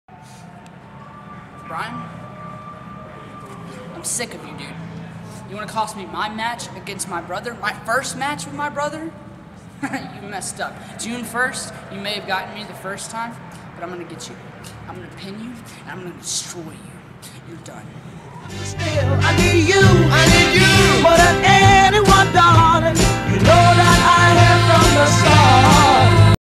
Marko Stunt is pissed off and is looking for vengeance vs Bryan Alvarez at Black Label Pro June 1st.
The rematch from Big Trouble in Little Crown Point between Marko Stunt and Figure Four Online's  Bryan Alvarez June 1st Crown Point, IN at 'There's Something About Baarry' RDS Gym LIVE on Independent Wrestling.TV

Username: blacklabelpro